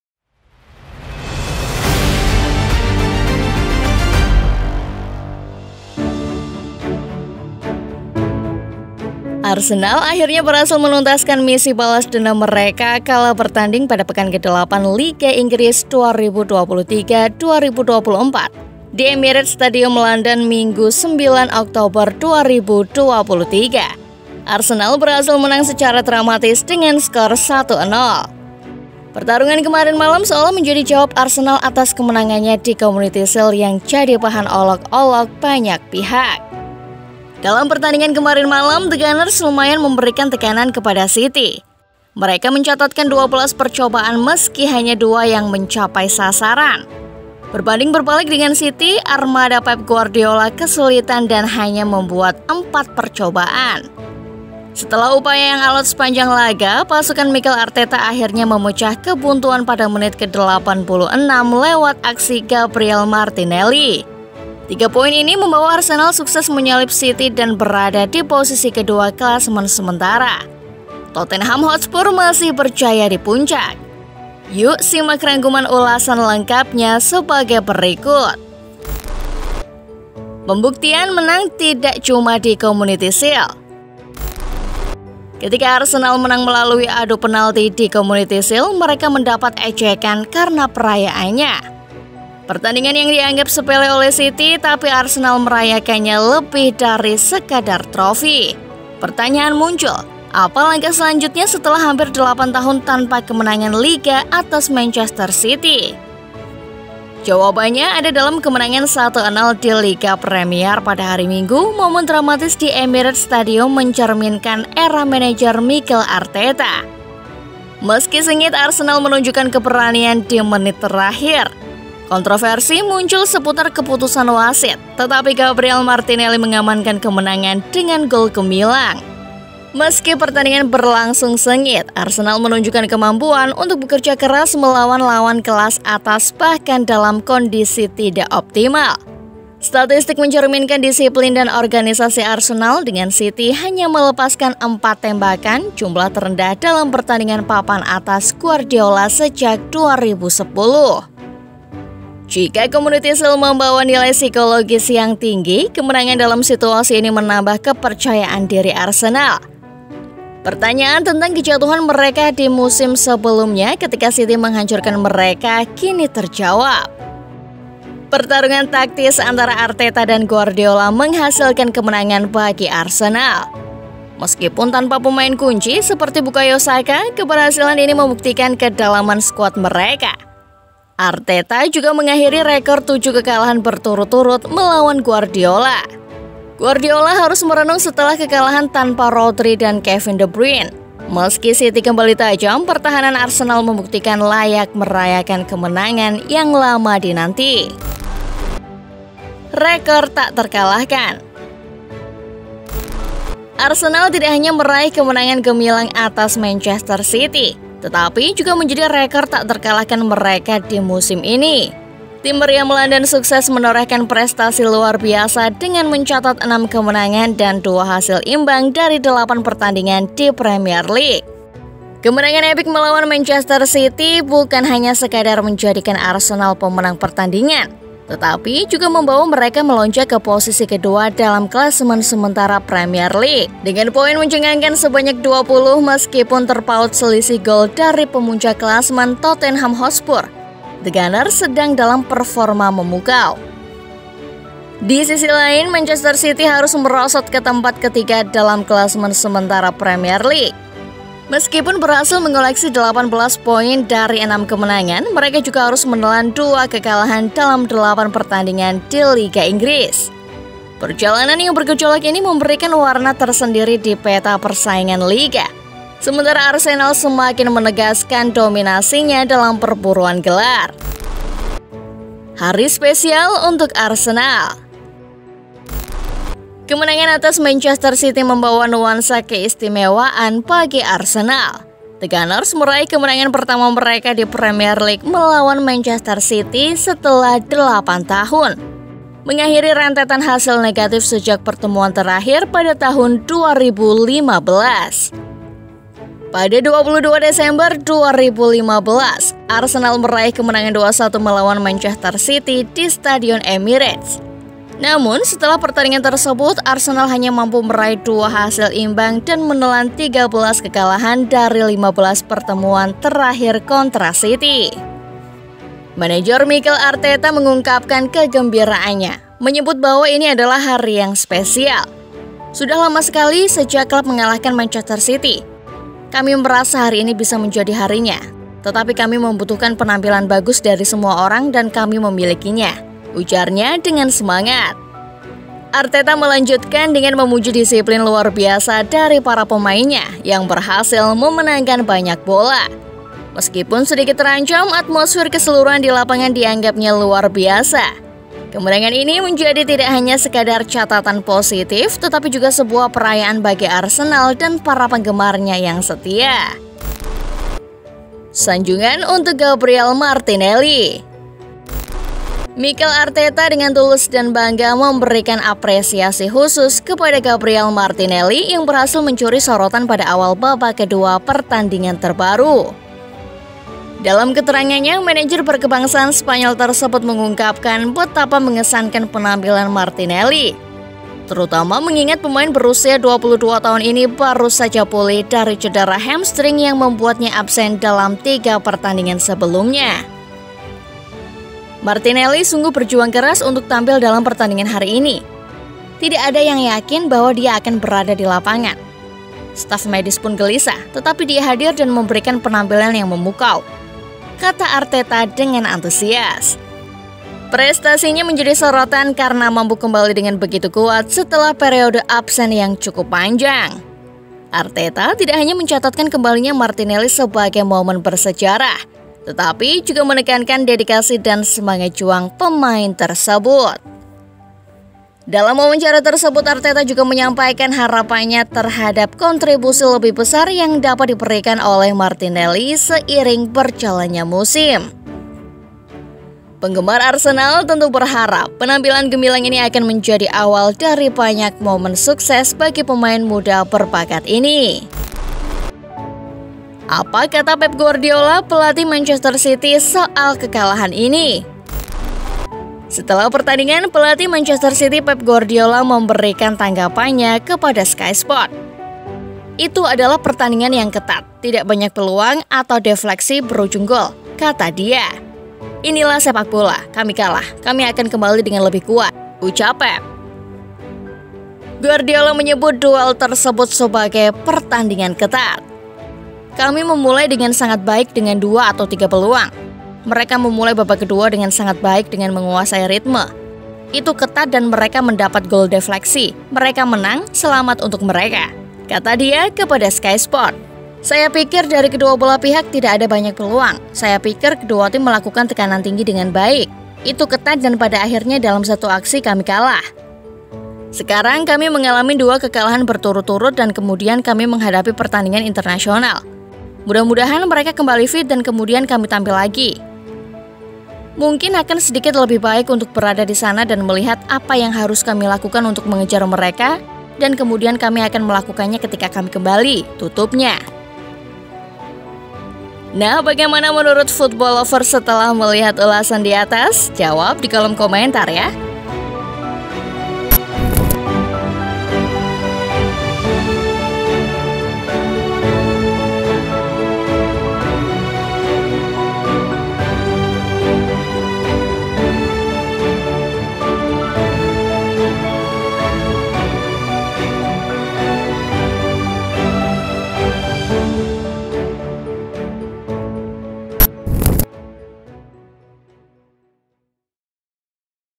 Bahan Cemoohan Yang Sekarang Makin Ngeri Dan Jadi Idola! Arsenal Kalahkan Man City Dengan Epik
Akhirnya Arsenal mampu balas dendam dengan epik atas Olok olokan dari Berbagai pihak, setelah mereka meraih juara di Community Sield dengan mangalahkan Man City.